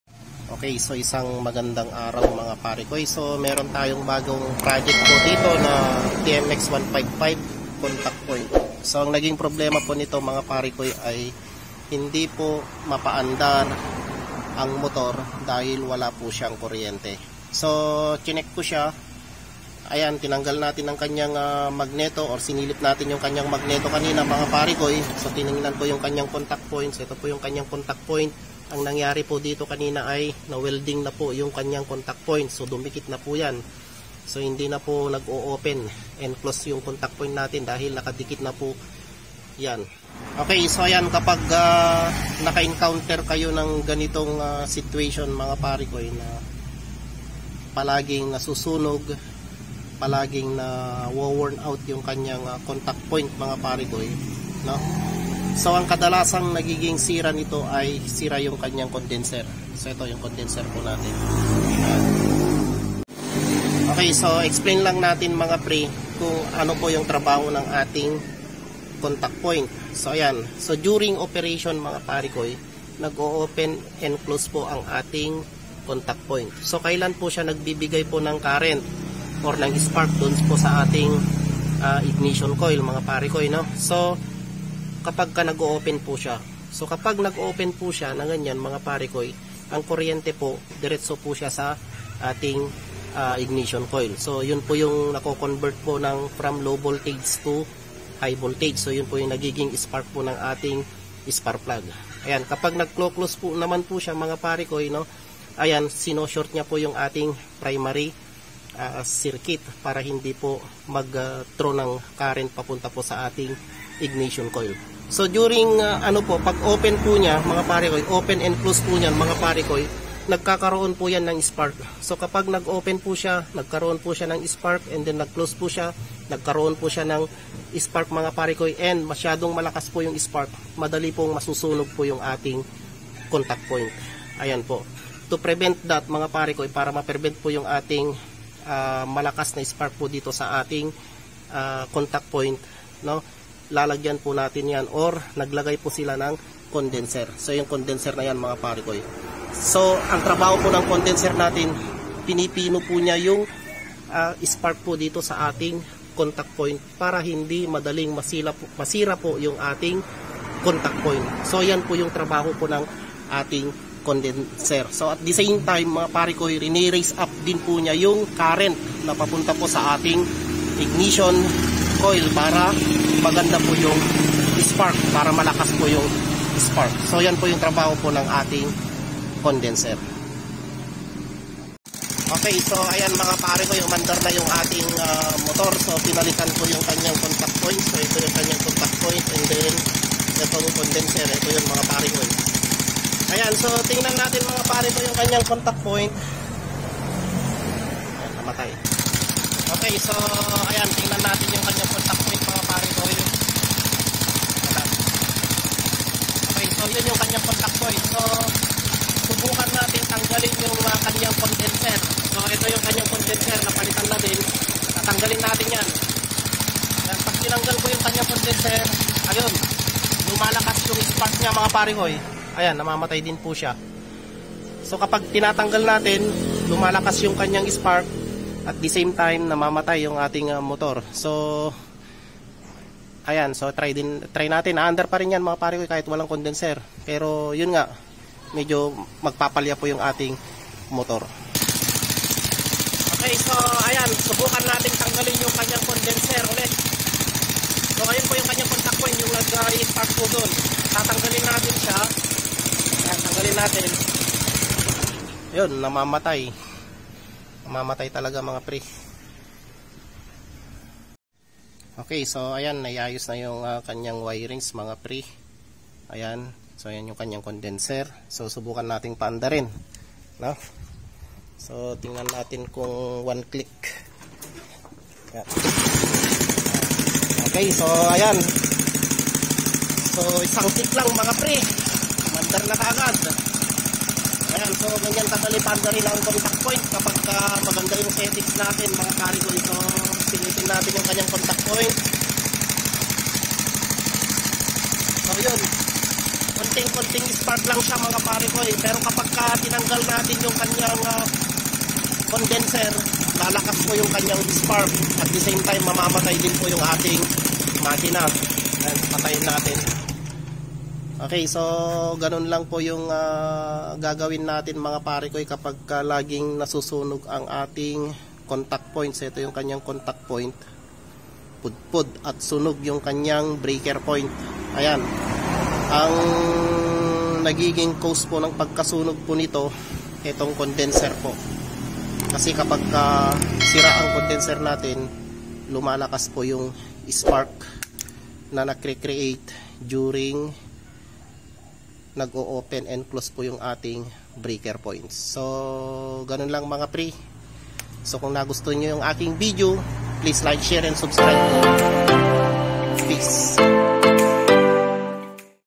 Contact point ng TMX 155 naweld at napudpud! Anu kaya ang possible cause nito?
Contact point ng tmx 155 naweld at napudpud! Ano kaya ang possible cause nito?
Samahan niyo po kami🥰
Pasupport po♥️